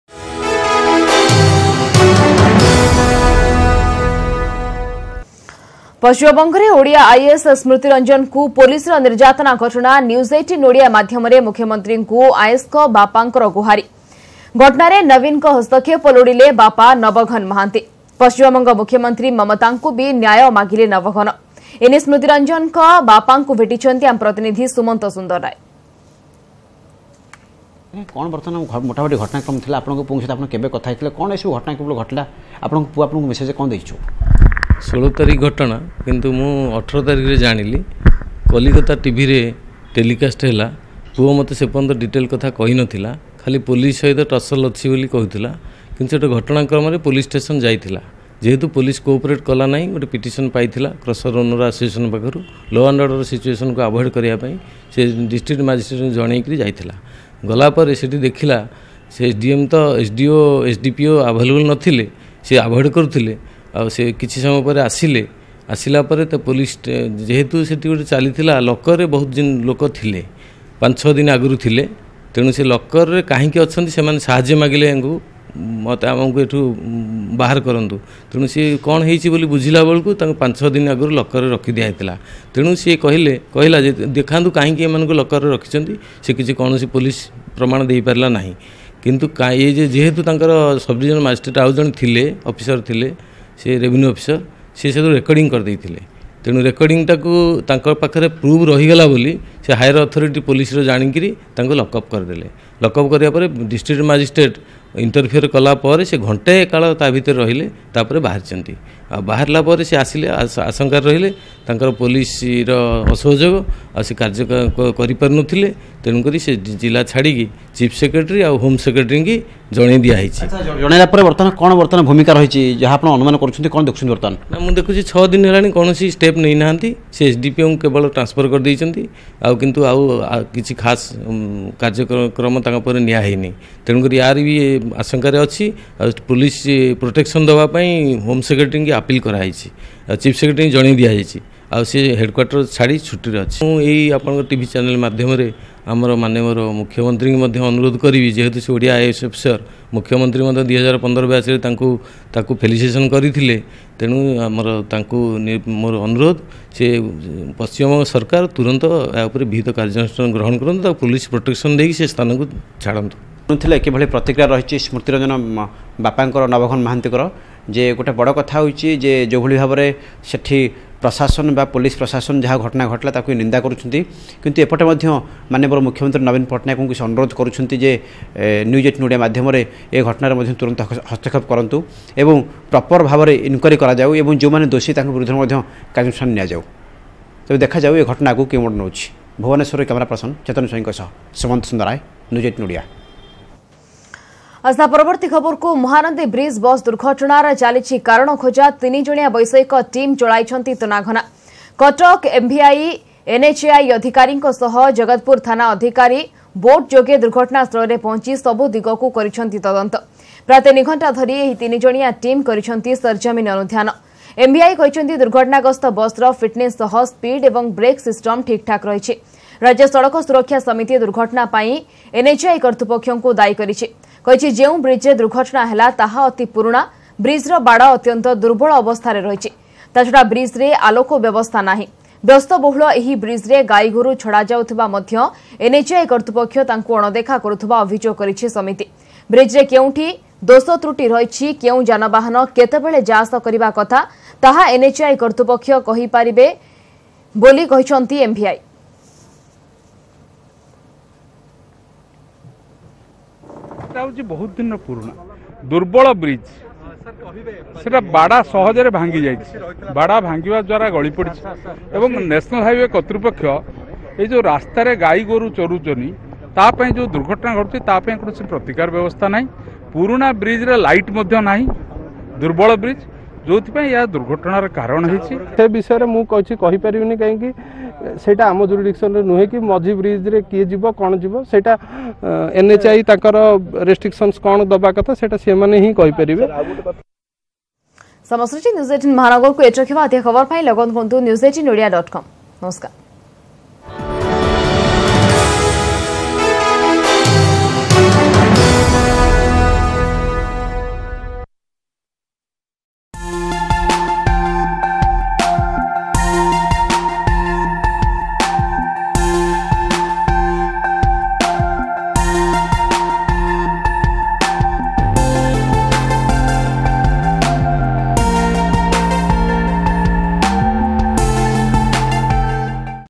ପଶ୍ଚିମବଙ୍ଗରେ ଓଡ଼ିଆ IAS କୁ ପୋଲିସ ର ନିର୍ଯାତନା ଘଟଣାରେ ସ୍ମୃତିରଞ୍ଜନଙ୍କ ବାପାଙ୍କ ସହ Exclusive Interview
ପଶ୍ଚିମବଙ୍ଗରେ ଓଡ଼ିଆ IAS କୁ ପୋଲିସ ର ନିର୍ଯାତନା ଘଟଣାରେ ସ୍ମୃତିରଞ୍ଜନଙ୍କ ବାପାଙ୍କ ସହ NEWS18 ର Exclusive interview . ଦେଖନ୍ତୁ କଣ କହୁଛନ୍ତି IAS ଙ୍କ ବାପା